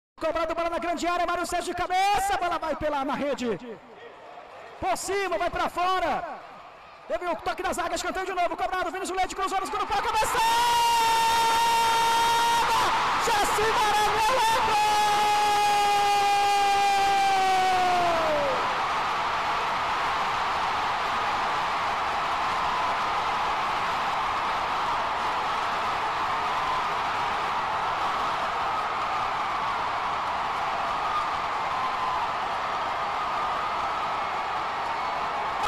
Gol Jacy Maranhão - Papão 1x0 Belo
Com um gol de Jacy Maranhão aos 55 minutos do segundo tempo, o Papão conseguiu mais uma vitória na Série C.